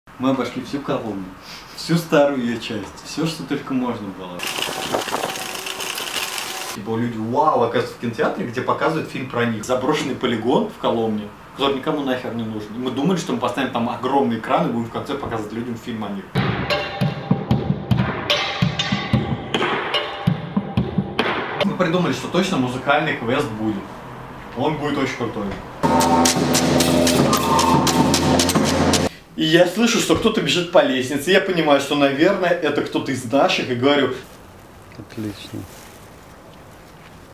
ТИЗЕР ЛЕКЦИИ "ПЕРЕАДРЕСАЦИЯ"
15 октября в 18:00 Мария Алигожина и Алексей Ермолаев -авторы проекта "ПЕРЕАДРЕСАЦИЯ" выступят в Цехе анимации ЦТИ ФАБРИКА

"ПЕРЕАДРЕСАЦИЯ» — проект, за два месяца превративший город в галерею, дом 19 века в новую вселенную, дом бездомных в выставочное пространство. Арт-экспедиция, разработанная в «Арткоммуналке», с элементами арт-клининга и арт-мусоринга.